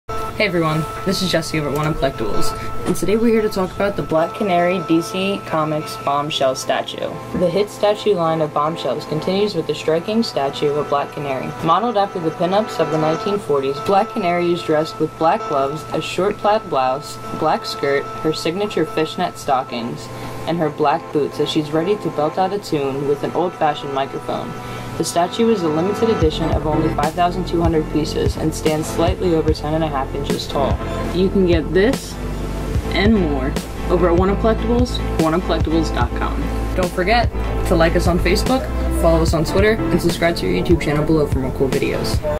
DC Comics Bombshells: Black Canary Statue
The hit statue line of Bombshells continues with this striking statue of Black Canary. Modeled after the pin-ups of the 1940's, Black Canary is dressed with black gloves, a short plaid blouse, black skirt, her signature fishnet-stockings, and her black boots as she's ready to belt out a tune with an old-fashioned microphone. The statue is a limited edition of only 5,200 pieces and stands slightly over 10 1/2-inches tall.